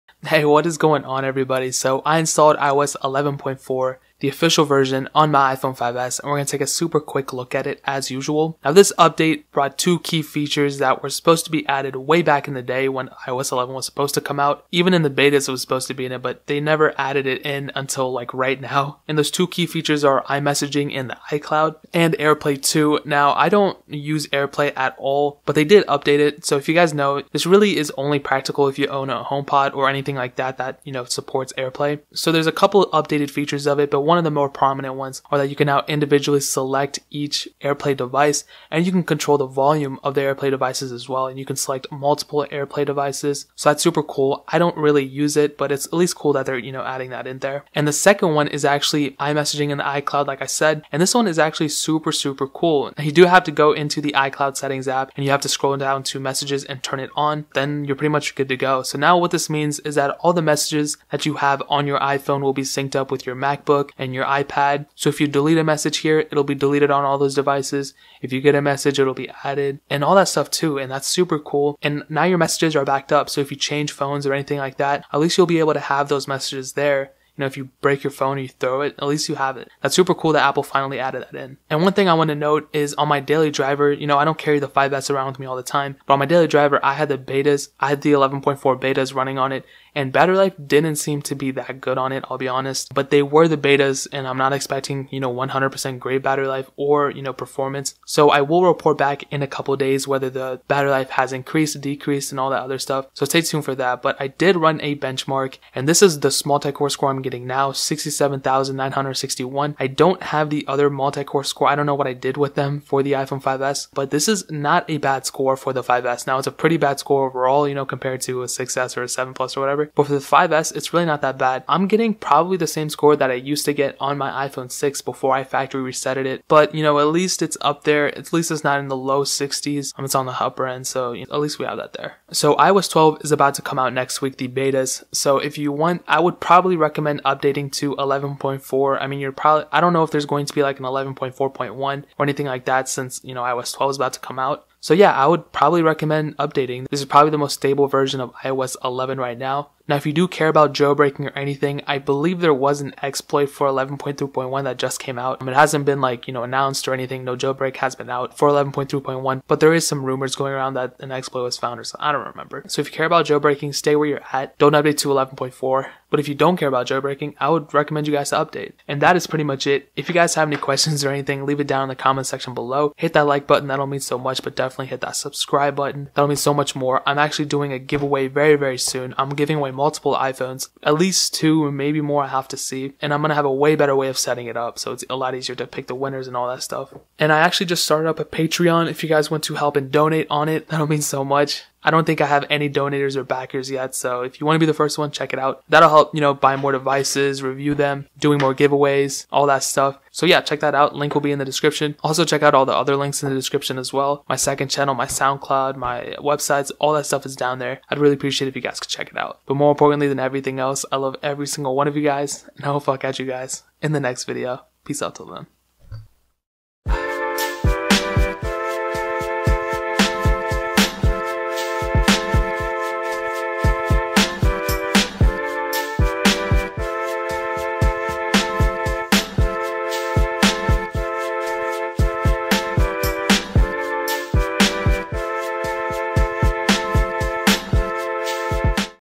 iOS 11.4 OFFICIAL On iPHONE 5S! (Review)
Phones I have laying around:
iPhone 8
iPhone 7
iPhone 7 Plus
iPhone SE
iPhone 6s

iOS 11.4 official just came out, so lets see how it runs on your iPhone!